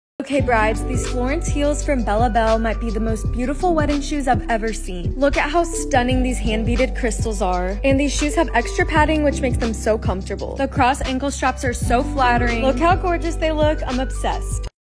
The most sparkly wedding shoes ever ✨
The “Florence” heels are the most beautiful and comfortable wedding shoes for the glamorous bride. Look at how they sparkle✨🤍 Would you wear these?